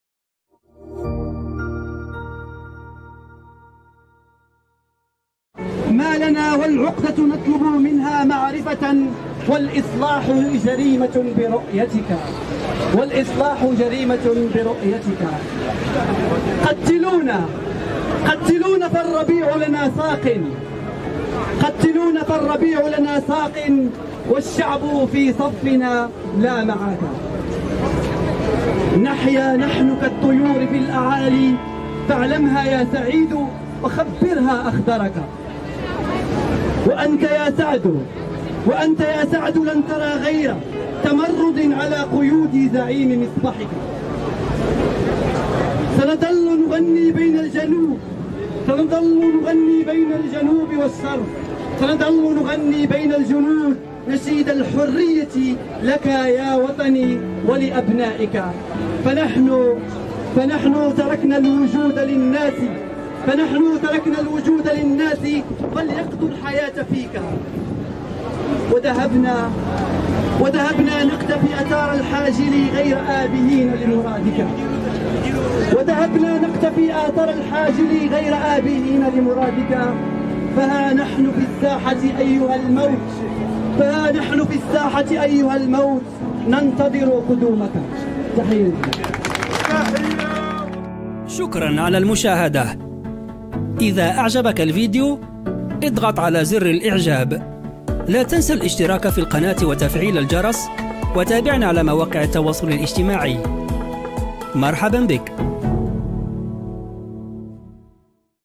قصيدة لأستاذ فرض عليه التعاقد من أمام اكاديمية الدارالبيضاء
2. اضغط على الجرس (🔔) بجانب زر الاشتراك
3. شارك مقاطع الفيديو مع أصدقائك وعائلتك
    يشرفنا اشتراكك بالقناة